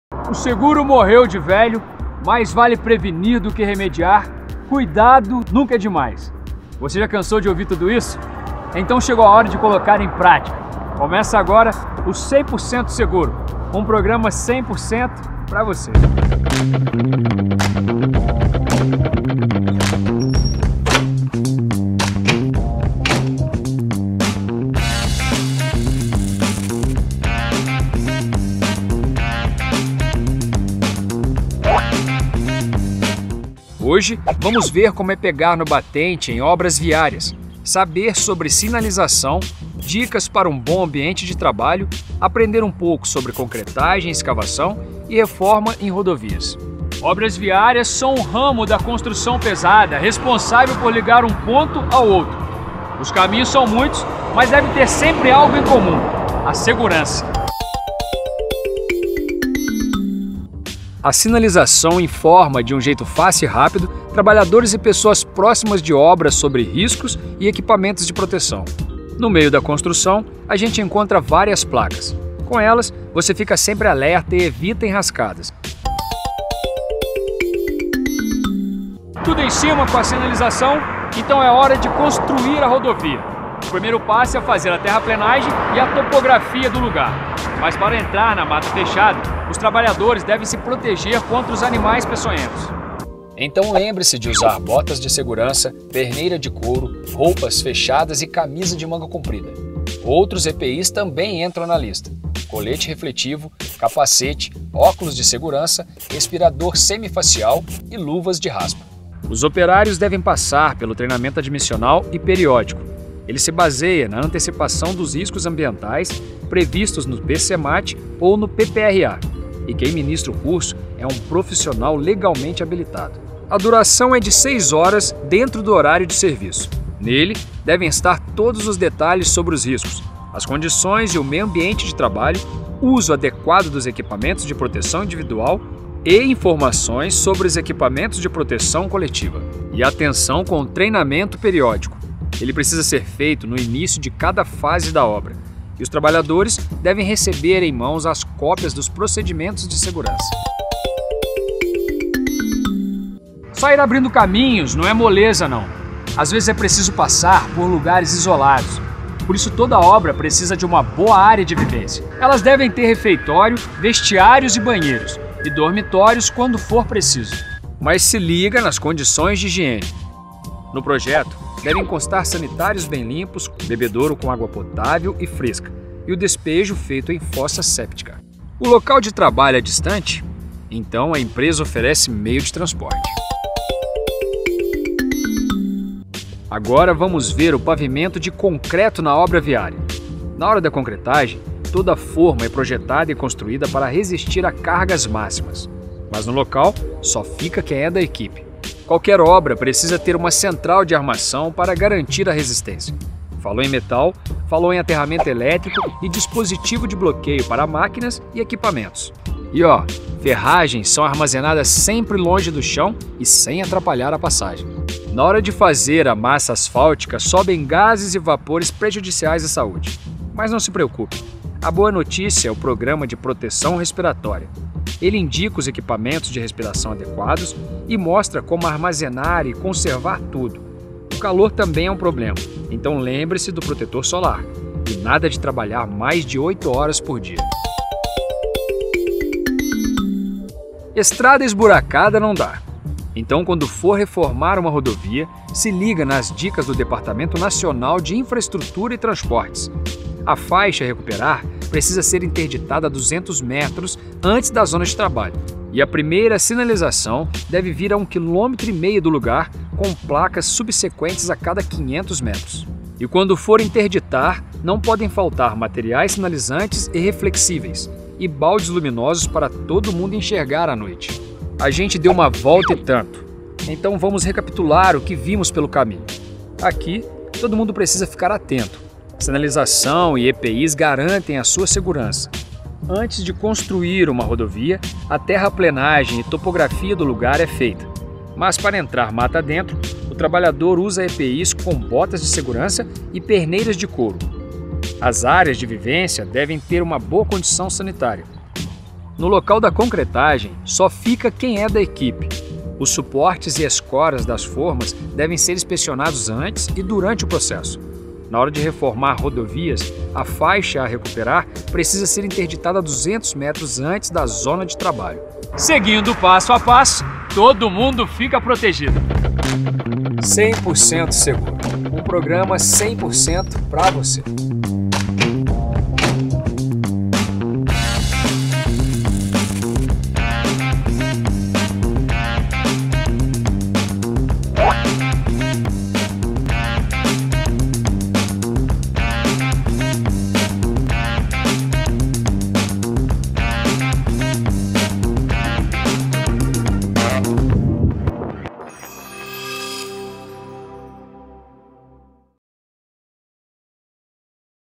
Projeto Série 100% Seguro | Segurança em Obras Viárias -- Parte 1 (Versão Completa)
Obras viárias são o ramo da construção pesada responsável por ligar um ponto ao outro de várias maneiras diferentes, mas sempre com segurança. Esta começa pela sinalização, que informa trabalhadores e pessoas próximas de obras sobre riscos e equipamentos de proteção. Várias placas diferentes alertam para evitar problemas: sinais de obrigação indicam os Equipamentos de Proteção Individual ou como agir; sinais de advertência apontam atividades arriscadas e precauções a tomar. Com a sinalização adequada, hora de construir a rodovia. Primeiro faça a terraplenagem e a topografia do lugar.
Observe que para entrar na mata fechada, você deve se proteger contra animais peçonhentos. Use botas de segurança, perneira de couro, roupas
fechadas, camisa de manga comprida, colete reflexivo, capacete, óculos de segurança, respirador semi-facial e luvas de raspa.